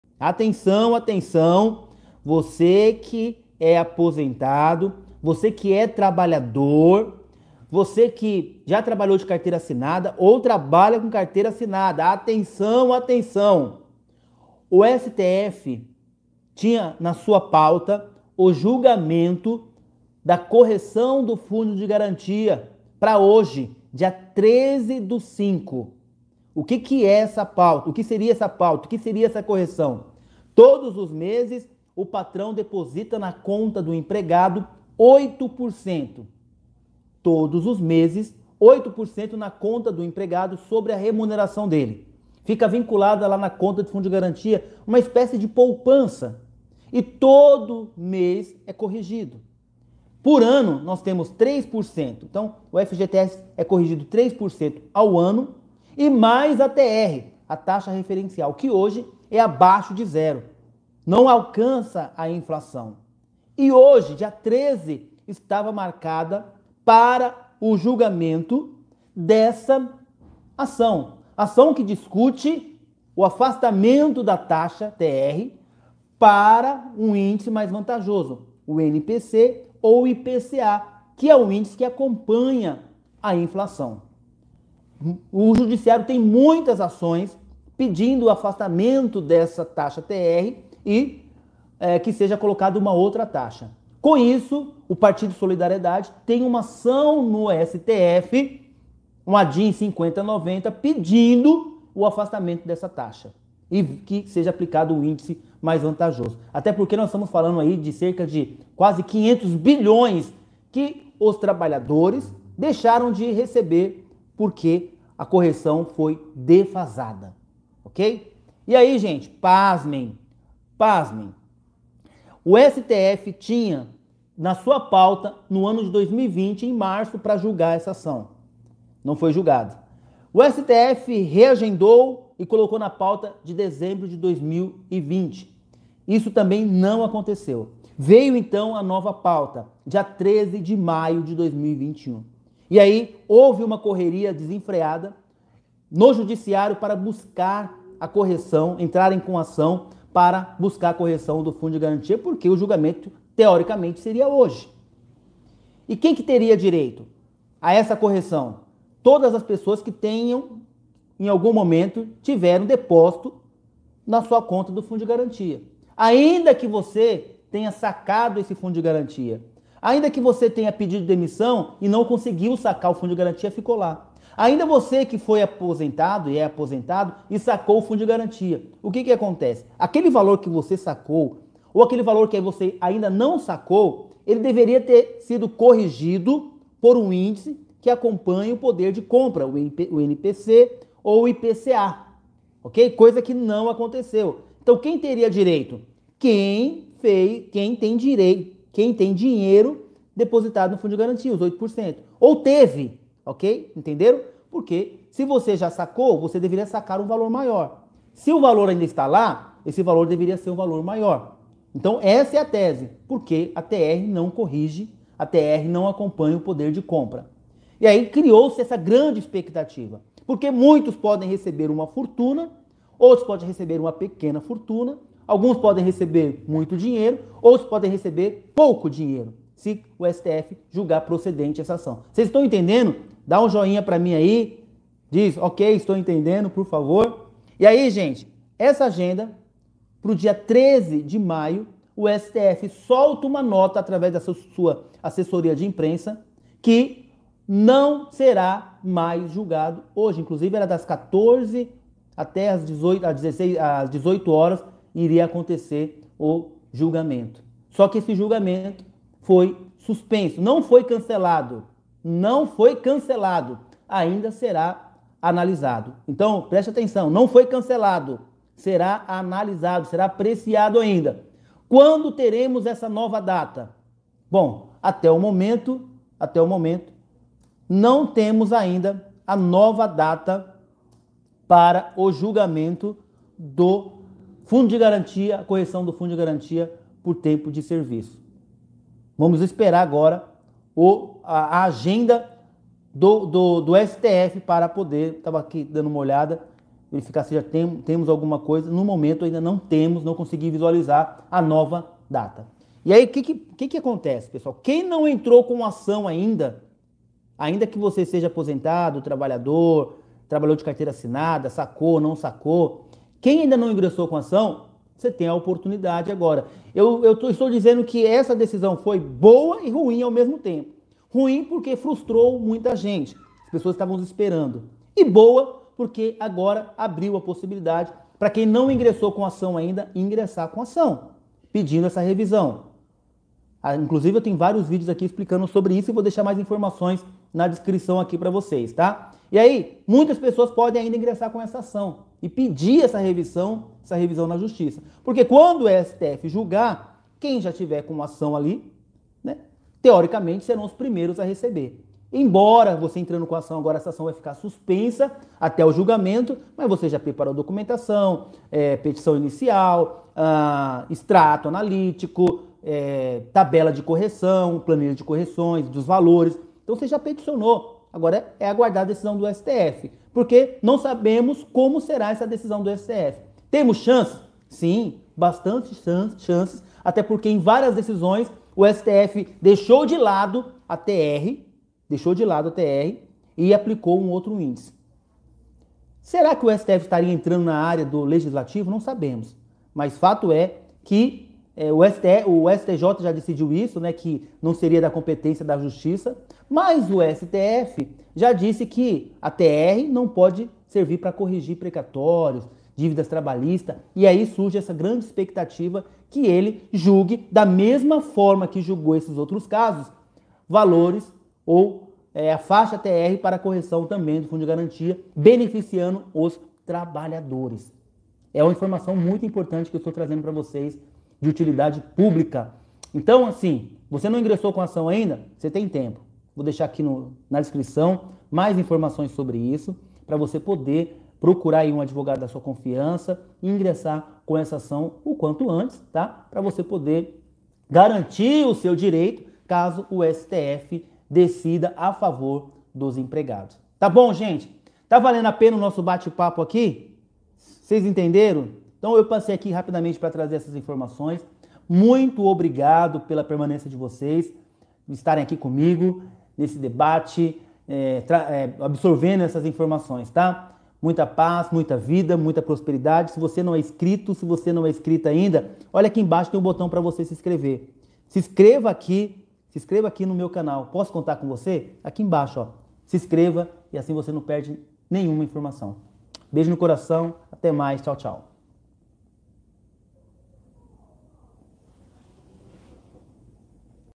MUDOU TUDO FGTS ! VOCÊ TRABALHOU REGISTRADO? SAIBA QUEM TERÁ DIREITO REVISÃO! PAUTA CANCELADA STF
Nesse vídeo iremos realizar mais uma atualização sobre a revisão do FGTS